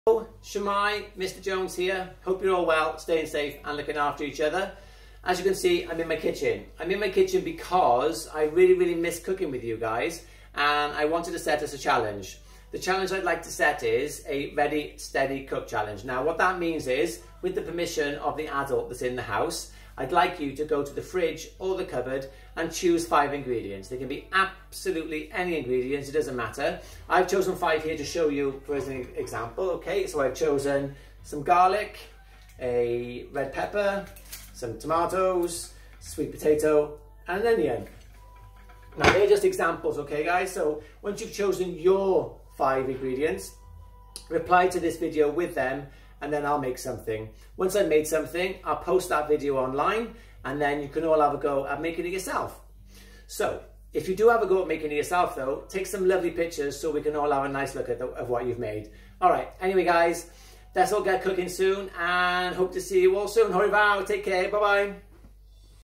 Woodlands Cooking Corner - Ready Steady Cook challenge with Mr Jones
Mr Jones shares a ready steady cook challenge with us!  Choose 5 ingredients you already have in your fridge or cupboard.  Send in your 5 ingredients via Twitter/Facebook/Instagram.  Mr Jones will choose someones ingredients and show us how to make something with them.